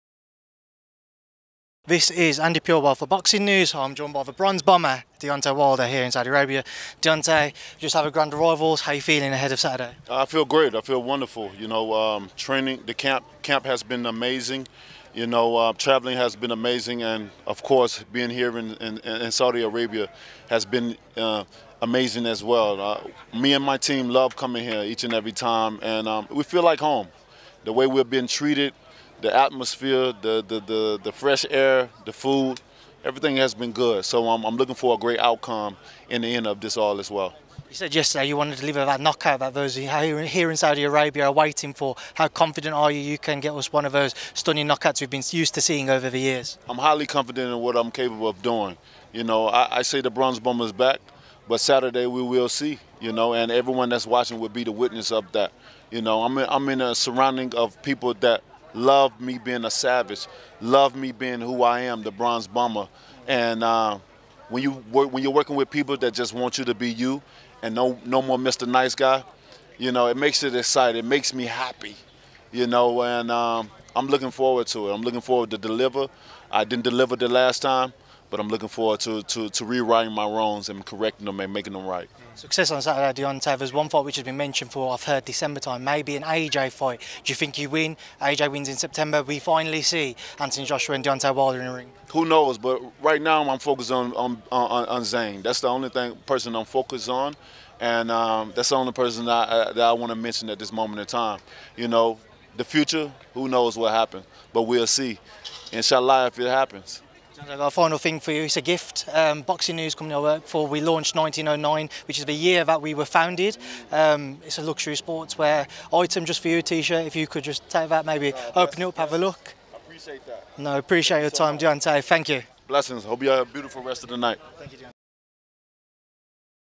Deontay Wilder On Anthony Joshua Fight & Vows To KO Zhang
Deontay Wilder speaks to Boxing News' Andi Purewal as discusses a potential Anthony Joshua fight in the future & vows to KO Zhilei Zhang.

00:00 - 00:08: Deontay Wilder intro
00:09 - 00:37: How he feels ahead of Saturday
00:38 - 01:23: Confident of KO win vs Zhang
01:24 - 01:47: Potential Anthony Joshua fight
01:48 - 02:05: 1909 gift
02:06 - 02:07: Deontay Wilder outro